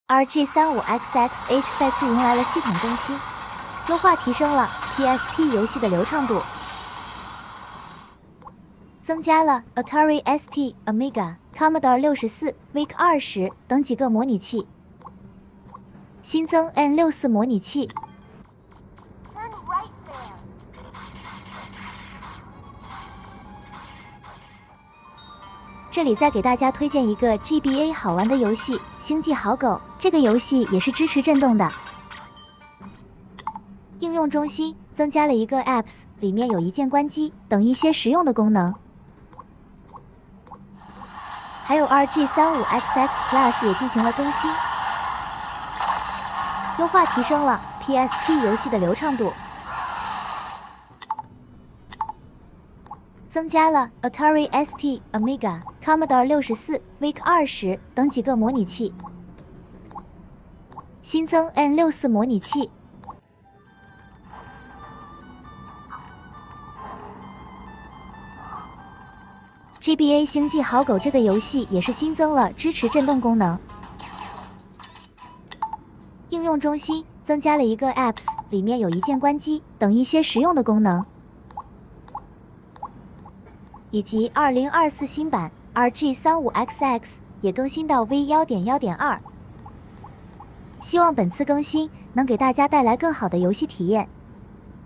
ANBERNIC RG35XX H/RG35XX Plus/2024 new version RG35XX system updated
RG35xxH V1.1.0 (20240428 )
Change log:
1. Optimize psp performance
2. Move apps to App Center
3. Add N64 emulator.
4. Add RA emulator ATARIST, AMIGA, AMIGA32CD, C16, C64, C128, VIC20
5. Update gpsp, mgba core to support interstellar good dog vibration
6. Add vba-next core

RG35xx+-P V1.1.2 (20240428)
Change log:
1. Optimize psp performance
2. Move apps to App Center
3. Add n64 power button to switch cross-key analog joystick
4. Add N64 emulator.
5. Add RA emulator ATARIST, AMIGA, AMIGA32CD, C16, C64, C128, VIC20
6. Update gpsp, mgba core to support interstellar good dog vibration
7. Add vba-next core

I hope this update will bring you a better experience!
Aliexpress store
Welcome to follow our official account,
Keep abreast of the latest information.

Anbernic:
INHERIT CLASSIC, CREATE FUTURE!